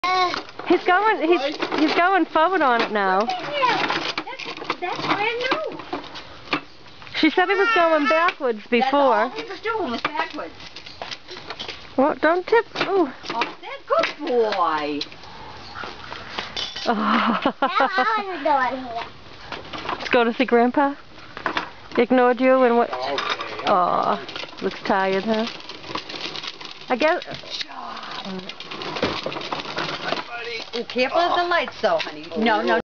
Learning to go forward on tricycle
A day with Nana 1.  Nana Paula and Grandpa drop by for a visit.